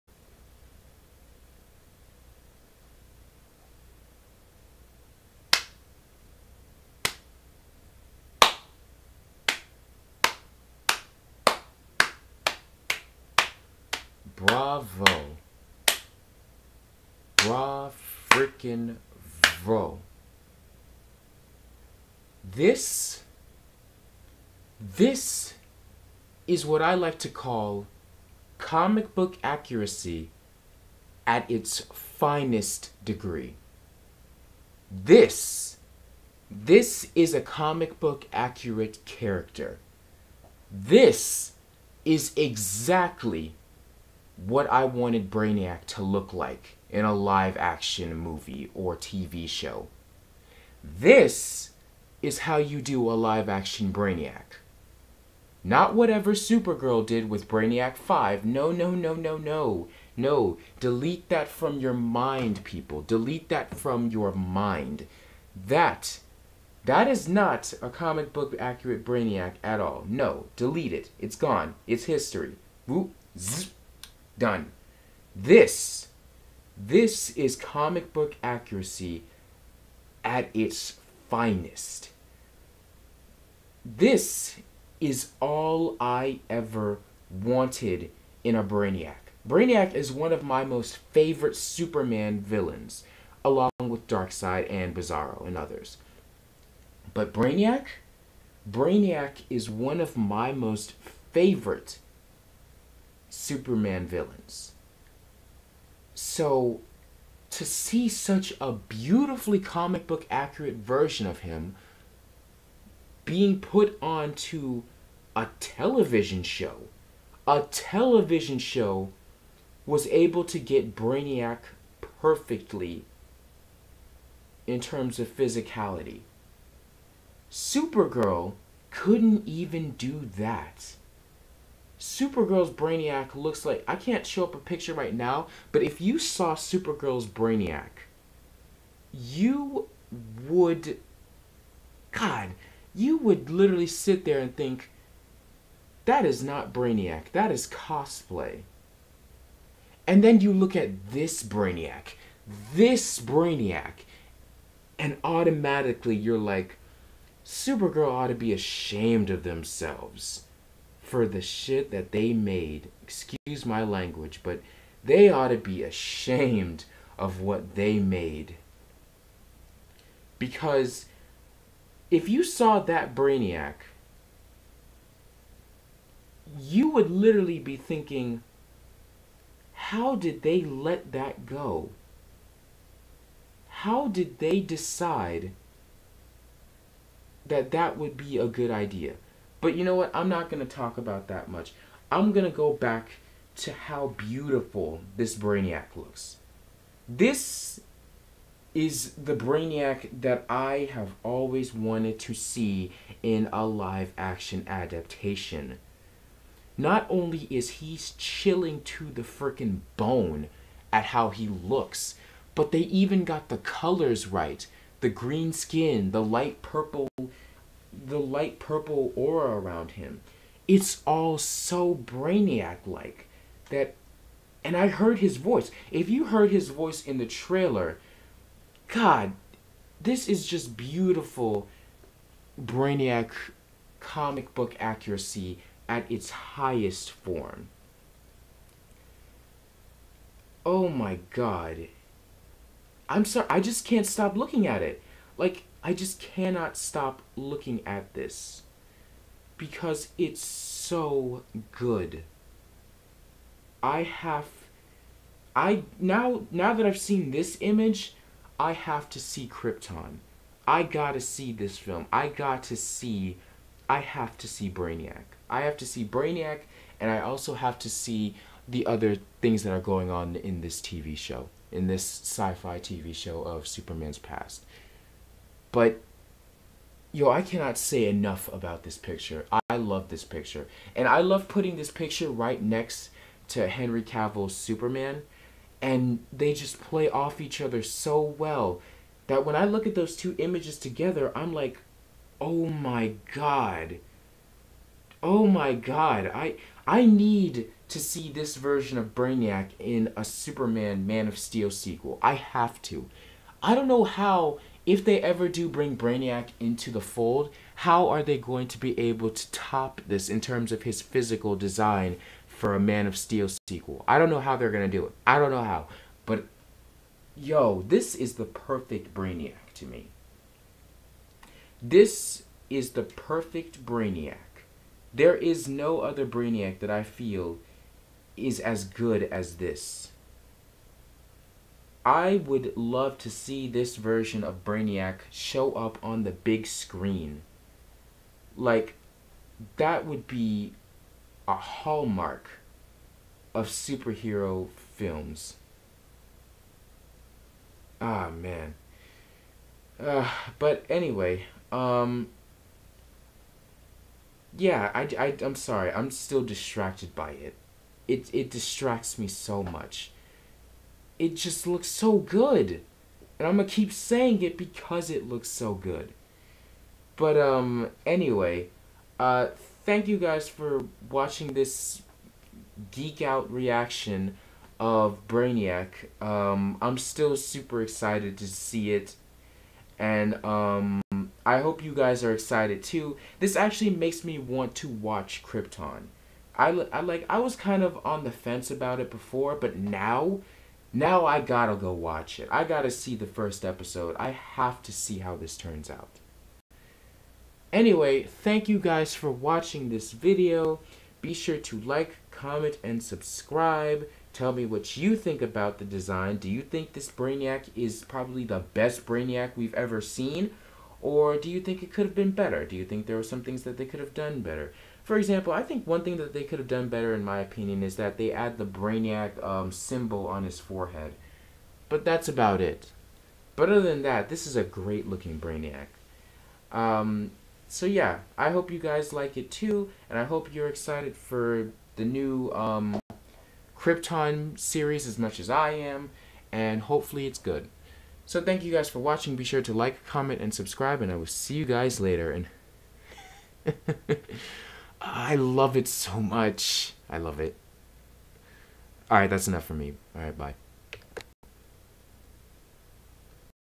FIRST LOOK// Brainiac image from SYFY's "Krypton" BEST BRAINIAC!
Now THIS is what I call comic book accuracy! It's like he came RIGHT out of the freakin' comics! Here are my thoughts on it!